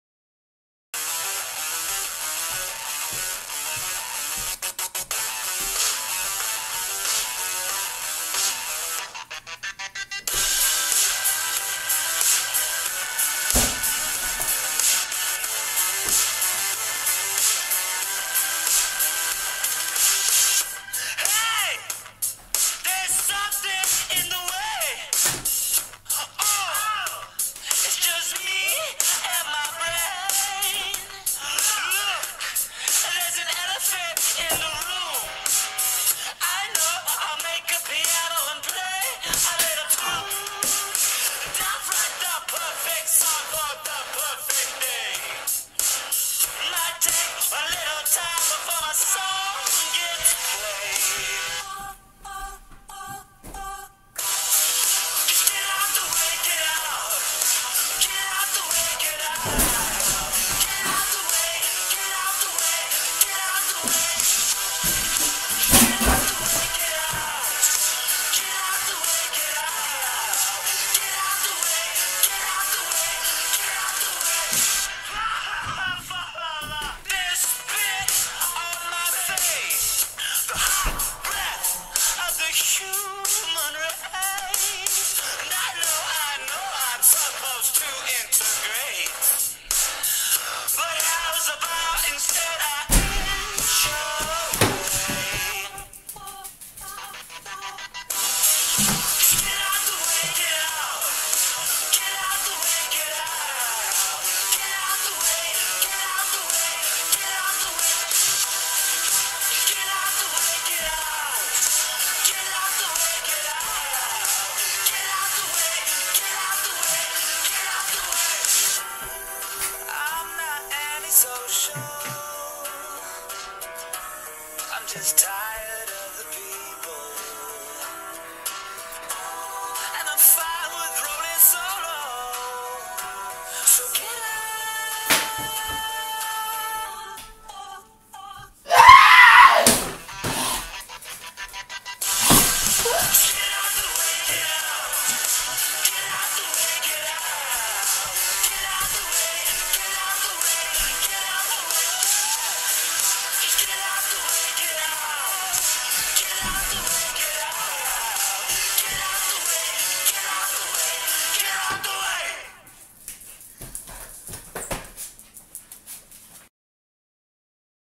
Dancing Taco Pt 2
I dance
I fall down
I dance some more
kiss me